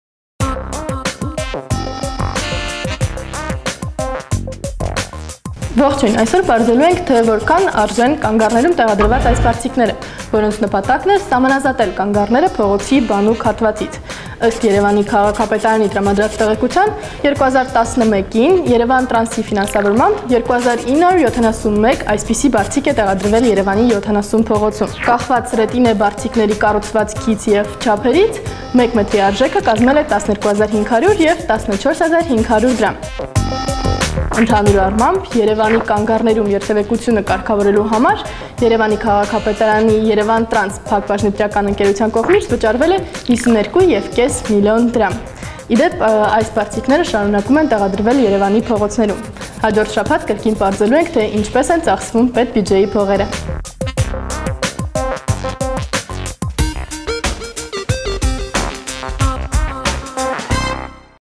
Քո գրպանից` կանգառներին
Որքա՞ն արժեն կանգառները փողոցի բանուկ մասից սահմանազատող ռետինե բարձիկները: